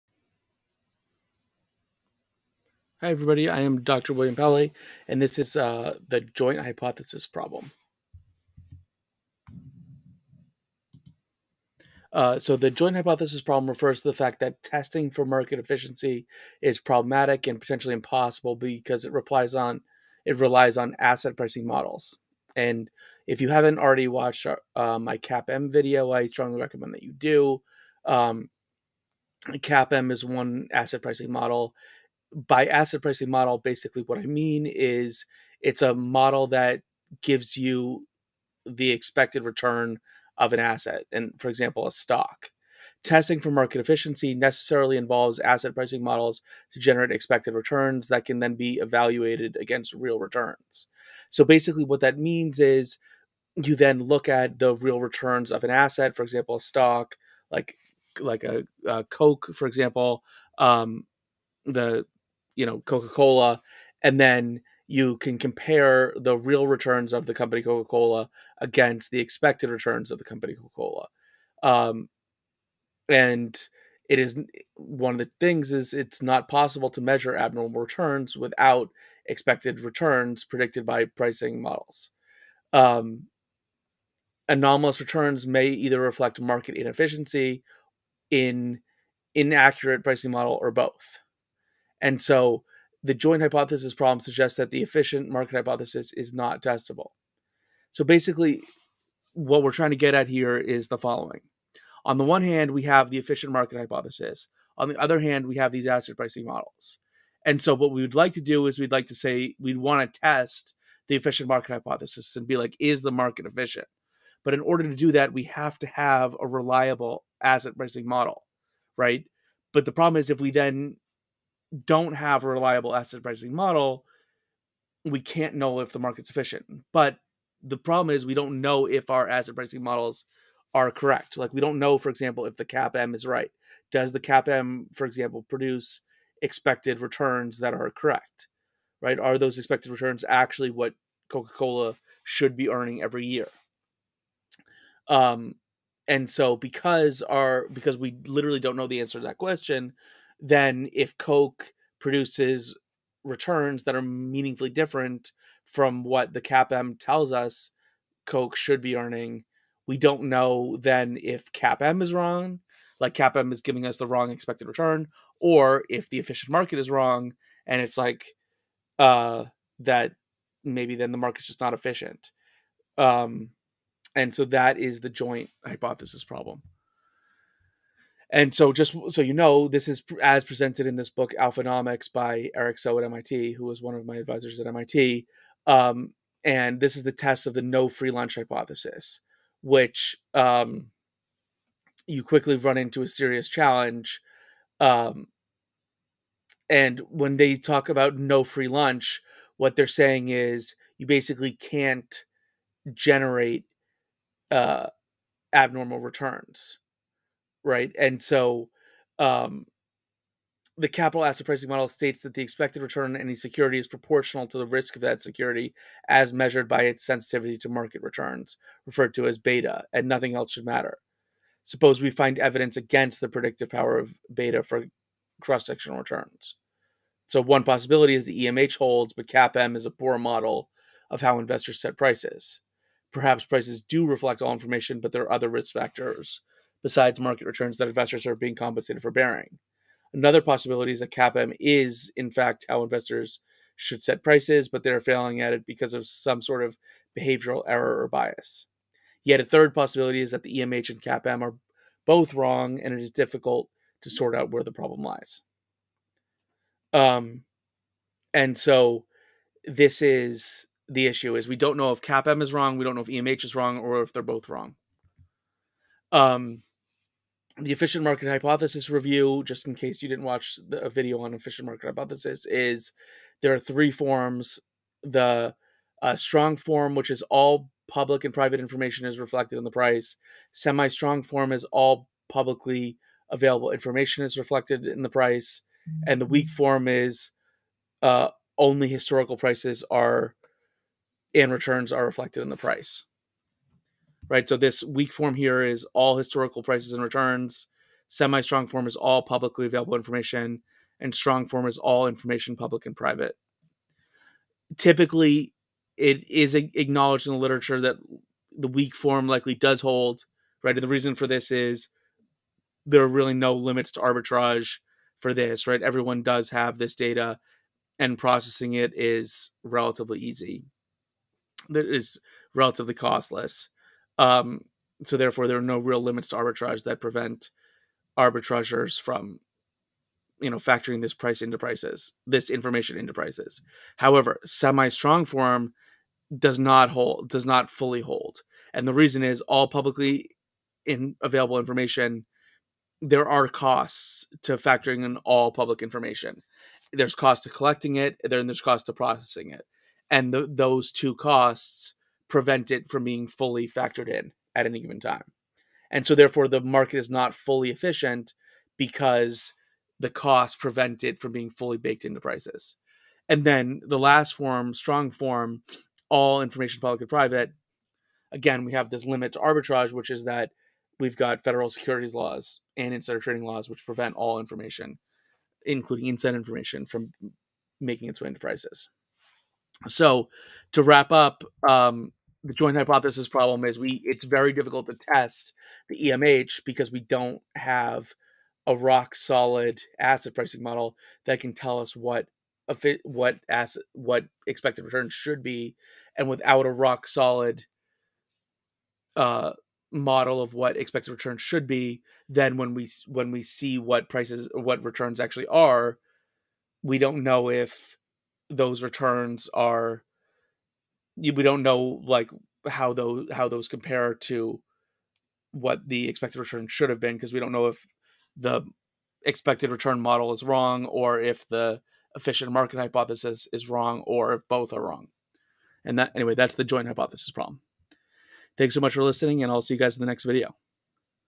Joint Hypothesis Problem
This video describes the joint hypothesis problem in asset pricing.